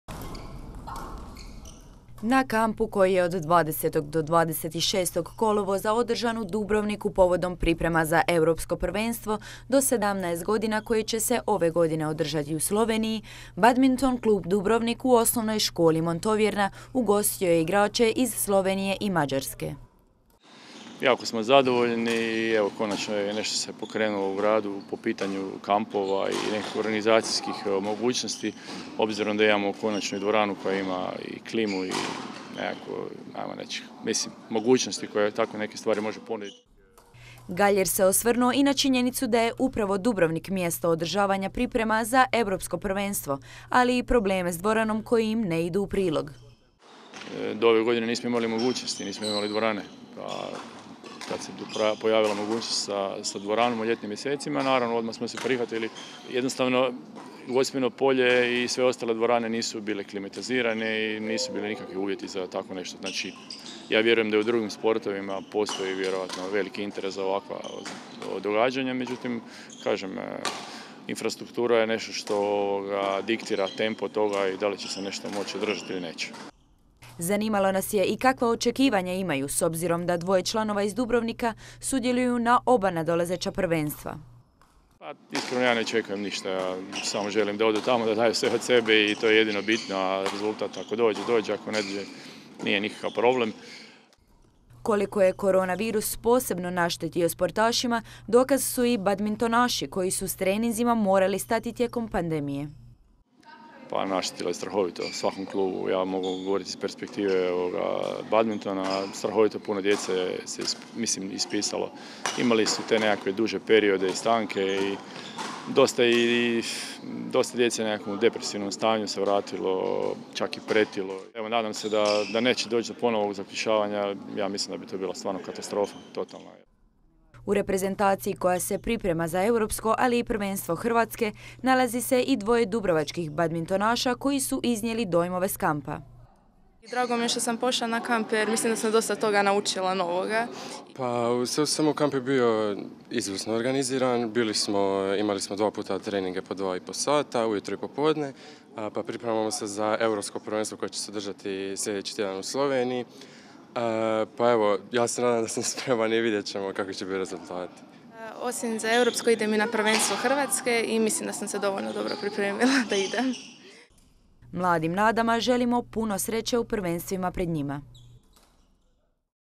ODRŽAN BADMINTON KAMP POVODOM PRIPREMA ZA EUROPSKO PRVENSTVO
DUTV Održan je posljedni dan regionalnog badminton kampa u organizaciji dubrovačkog badminton kluba koji je ugostio reprezentacije Slovenije i Mađarske. Provjerili smo jesu li spremni za prvenstva koja su pred njima, te kako su protekle pripreme.